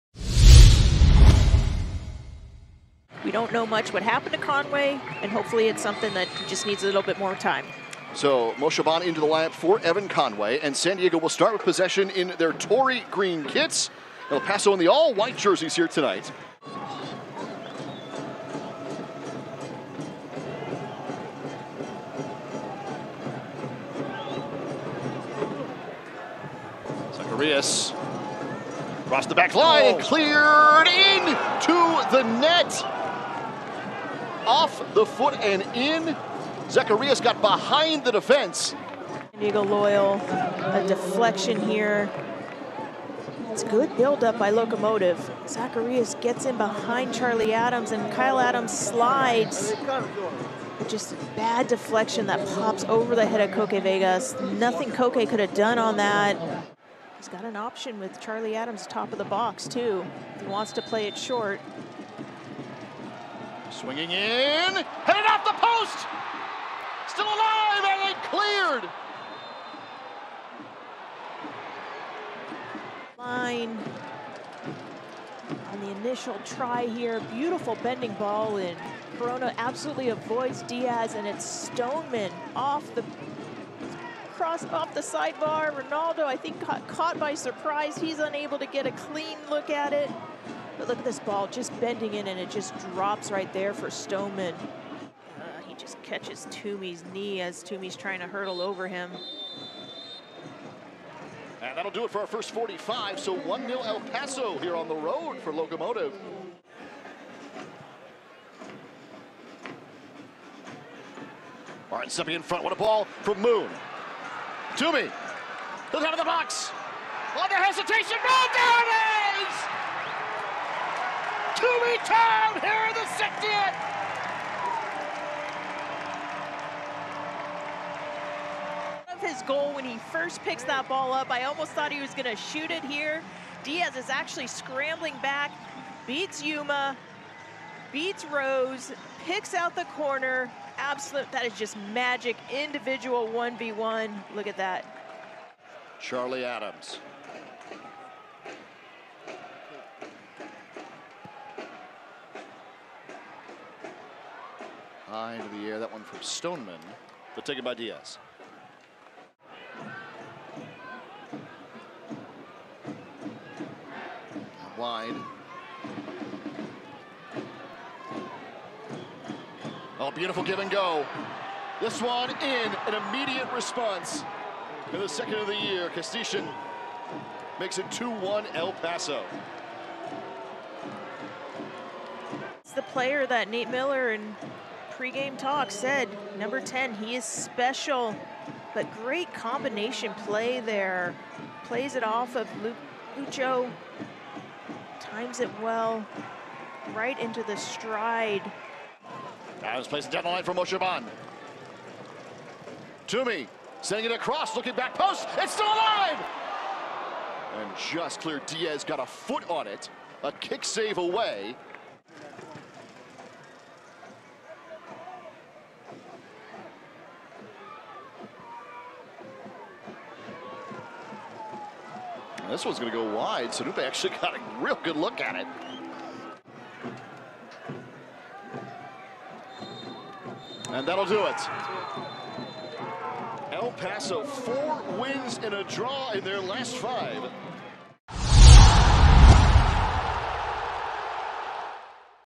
4.29.2023 | San Diego Loyal SC vs. El Paso Locomotive FC - Game Highlights
Denys Kostyshyn scored the decisive goal as El Paso Locomotive FC took a 2-1 victory against San Diego Loyal SC on Saturday night, becoming the first visitor to win at Torero Stadium this season while extending its undefeated streak to five games.

00:00:00 - Kick Off by San Diego Loyal SC
00:00:17 - Goal by Ricardo Zacarías
00:00:57 - Shot by Grant Stoneman
00:01:35 - End Period by San Diego Loyal SC
00:01:47 - Goal by Robinson Moshobane
00:02:26 - Shot by Grant Stoneman
00:02:39 - Goal by Denys Kostyshyn
00:03:19 - GK Save by Benny Díaz
00:03:32 - DefensiveAct by Elijah Martin
00:03:36 - Shot by Emmanuel Sonupe
00:03:46 - End Match by El Paso Locomotive